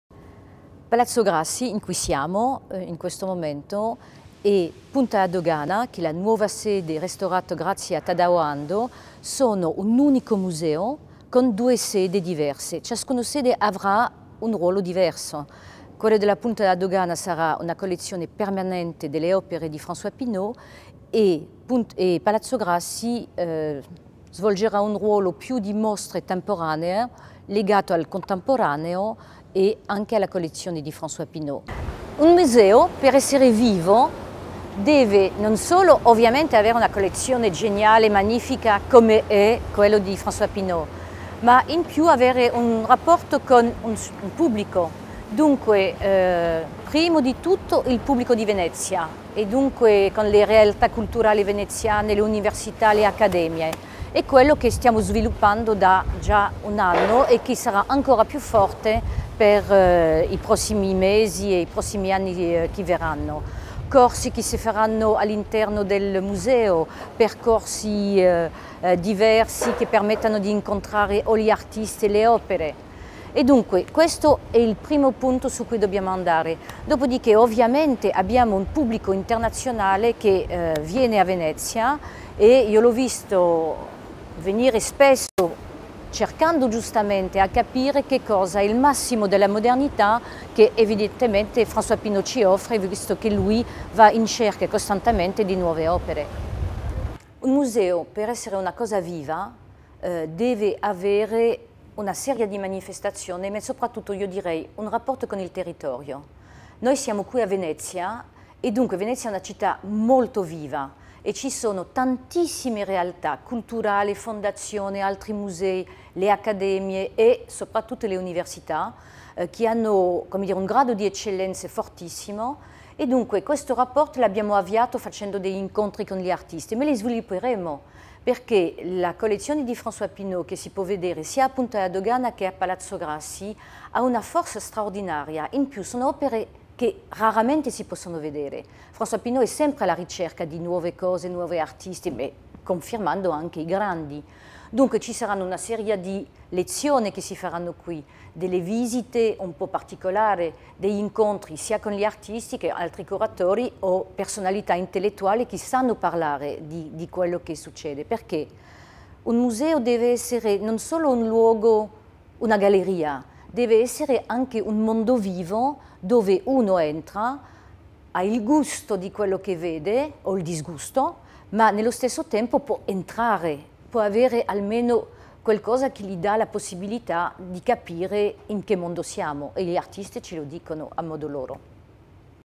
Mapping the Studio - Monique Veaute
La direttrice di Palazzo Grassi, Monique Veaute, presenta la mostra Mapping the Studio: Artists from the François Pinault Collection